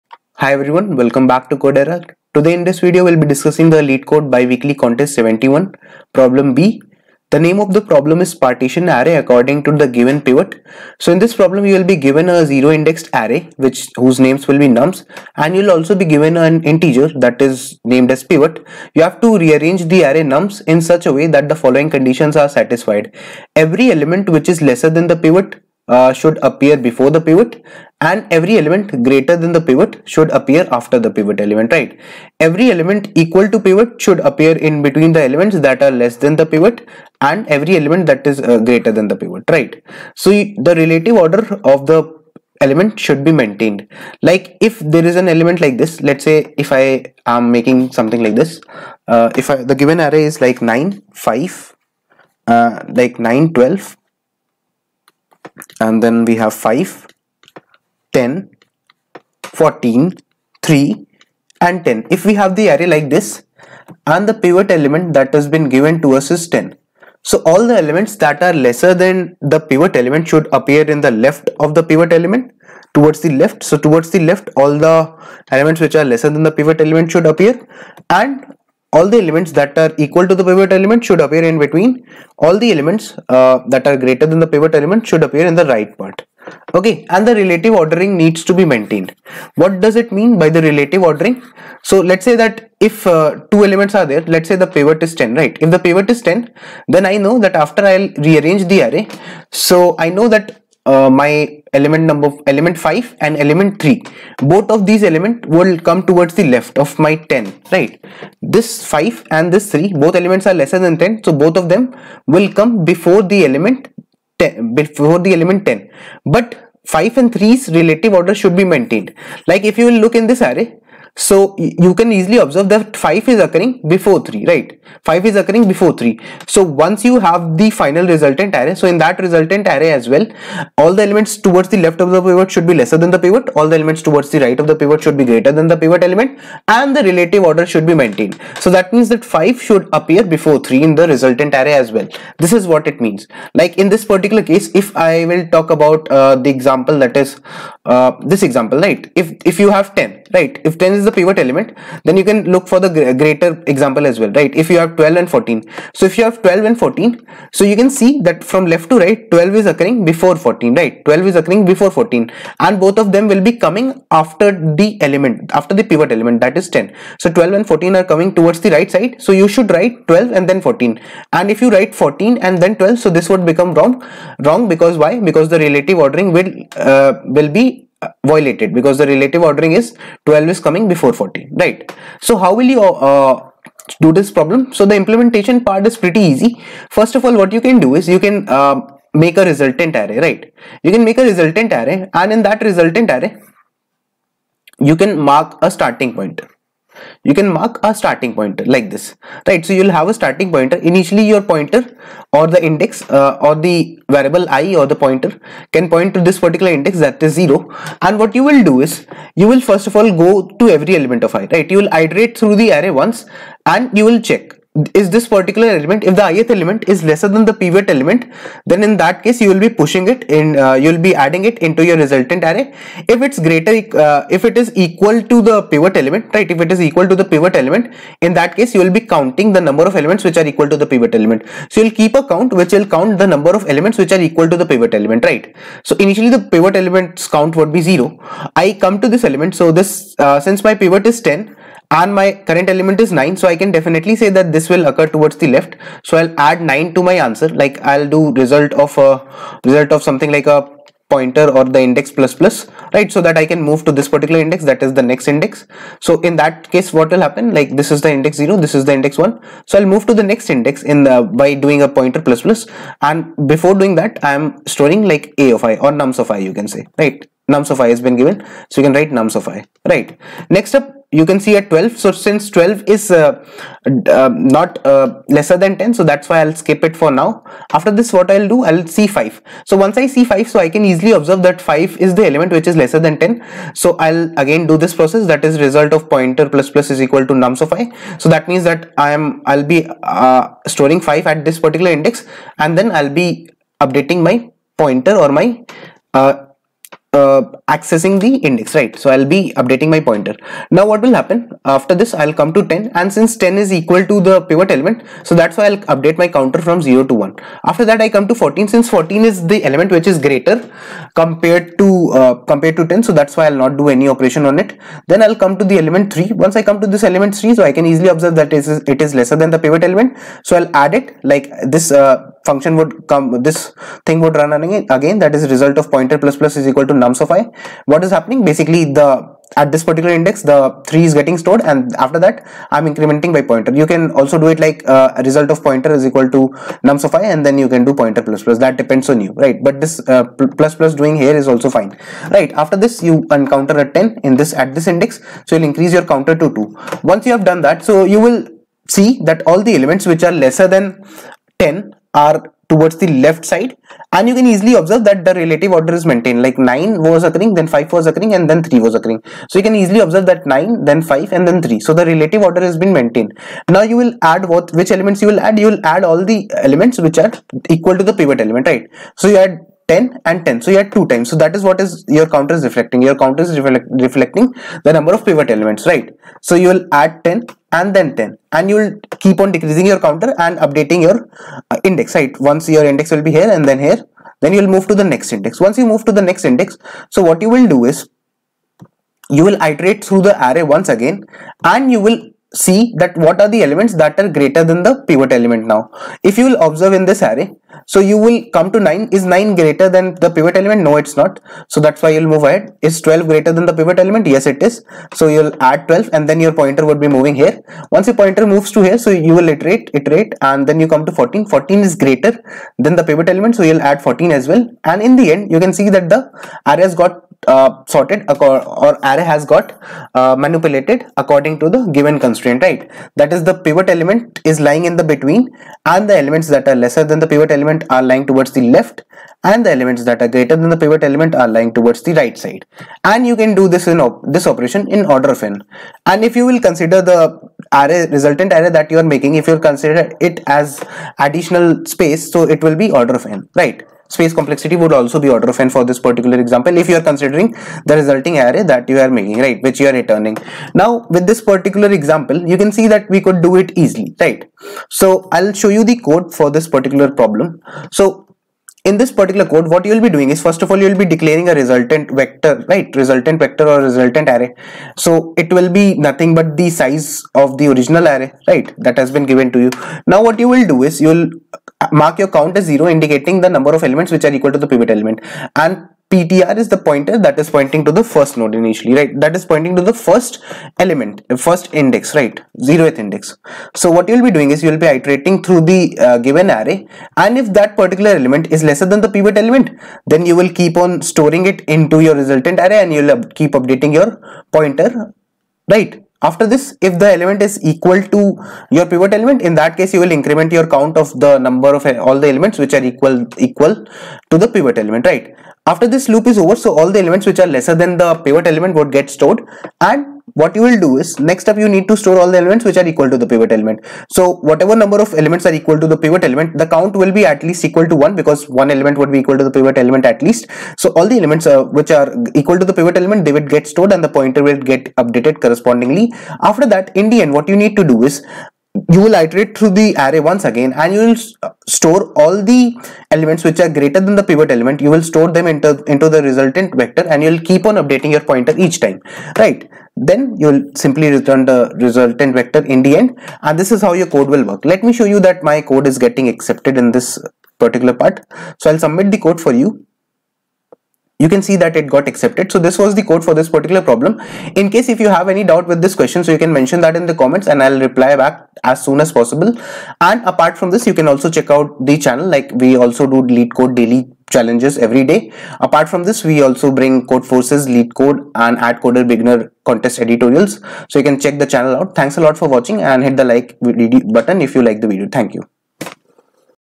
Partition Array According to Given Pivot | LeetCode Biweekly Challenge 71 | Yash Dwivedi
Time Stamps:
0:00 Understanding Problem Statement
03:08 Approaching the solution
7:39 Implementing the Code